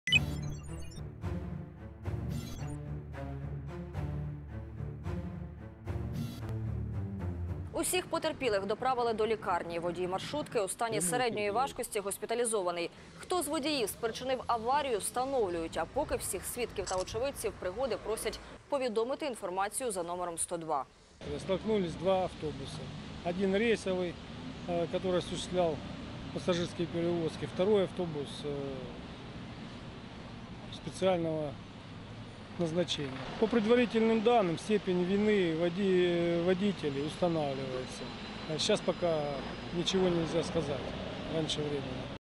У Маріуполі маршрутка зіштовхнулась із автобусом військових
Постраждали 12 людей, з них четверо військові, інші – цивільні. Усіх потерпілих доставили до лікарні. Водій маршрутки – у стані середньої важкості госпіталізований. Хто з водіїв спричинив аварію встановлюють, а поки всіх свідків та очевидців пригоди просять повідомити інформацію за номером 102.